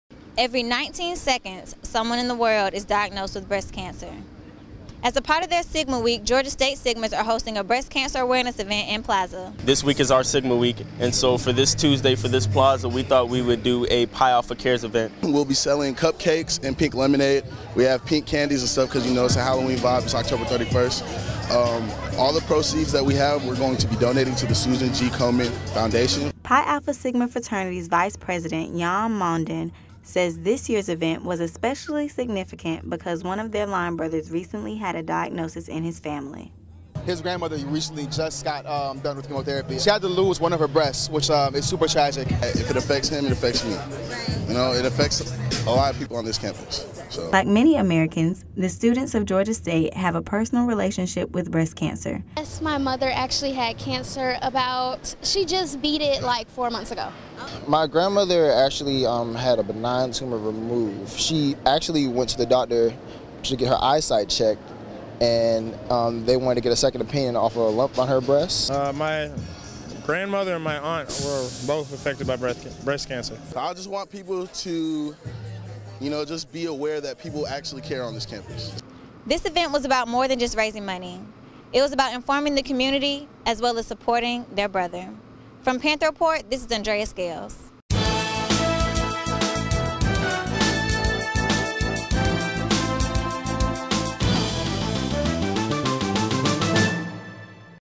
Phi Beta Sigma hosts Breast Cancer Awareness event
Georgia State Sigmas hosted a Breast Cancer Awareness Event during Sigma week. Reporter Andrea Scales has the details.

Reporter/Editor: Andrea Scales
Videographer: Cecil Hannibal